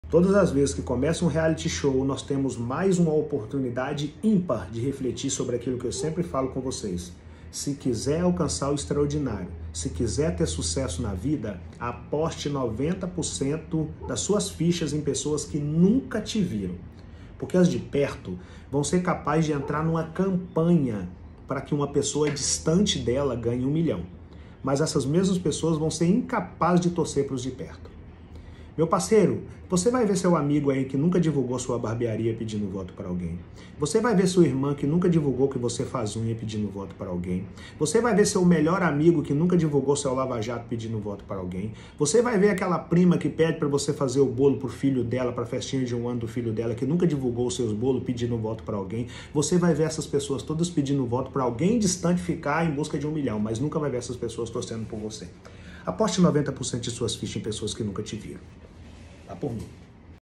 🚨JAMAIS DE ESQUEÇAM DA REGRA 08.
Aposte 90% de suas fichas em pessoas que nunca te viram, pois os de perto serão capazes de torcer para alguém distante, mas nunca irão divulgar o seu negócio.
Portanto, se quiser deslanchar seu negócio, conheça novas pessoas TODOS OS DIAS.

O papo precisa ser reto e sem curva.